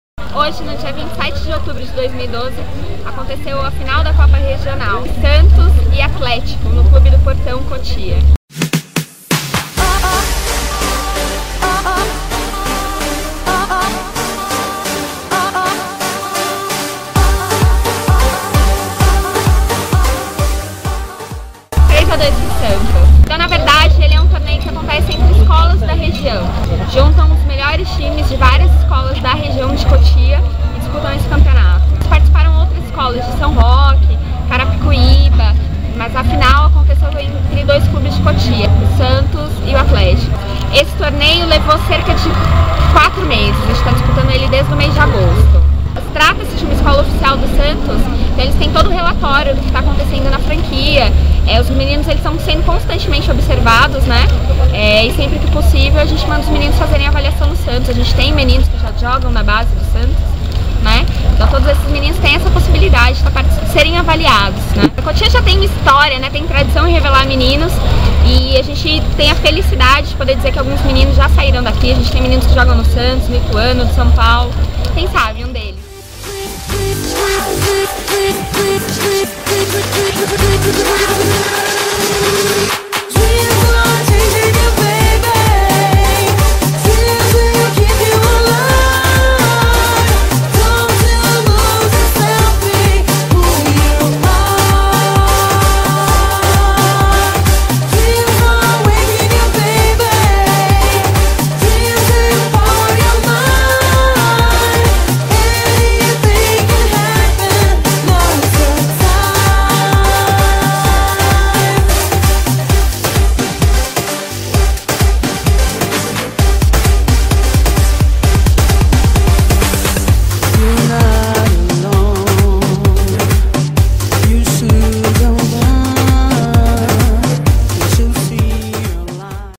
Futebol em Cotia - parte 2
Decisão da Copa Inter Escolas de Futebol com Leo.